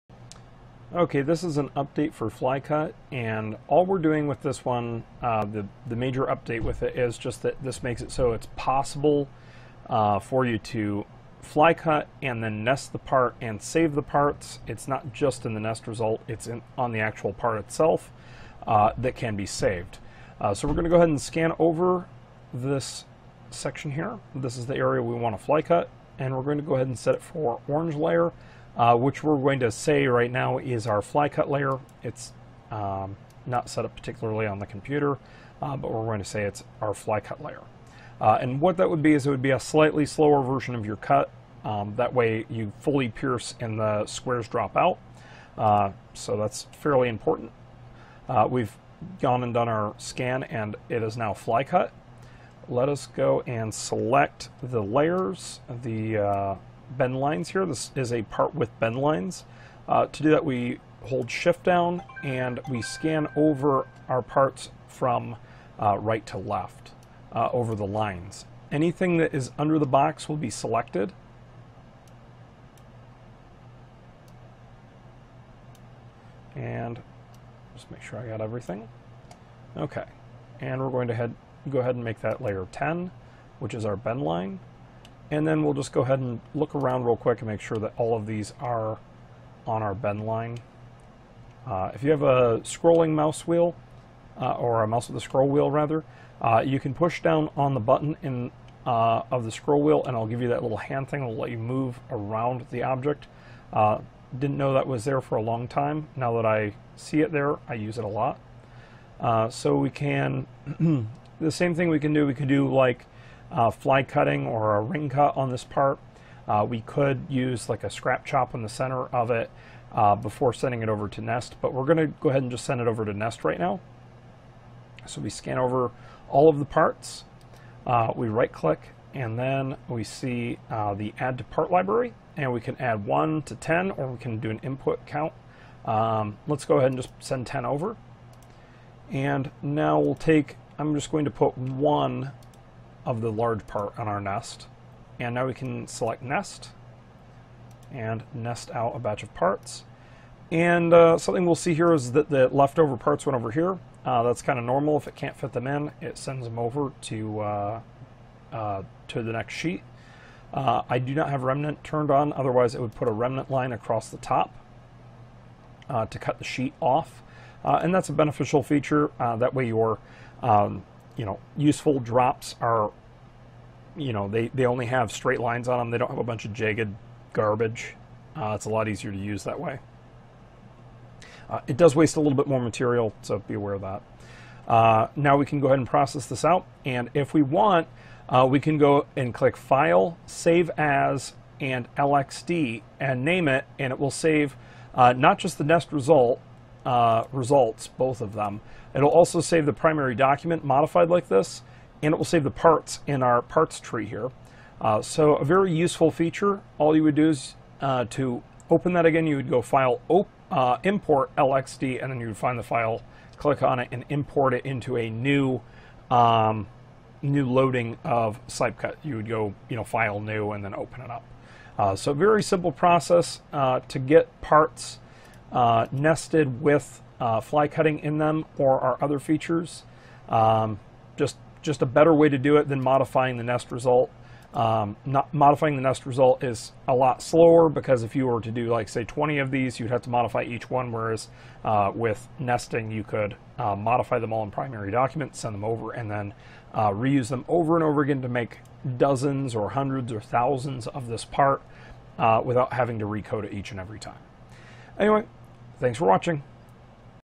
Nesting fly cut parts in cypcut.
Update video showing setting up parts with flycutting that will be nested with other parts.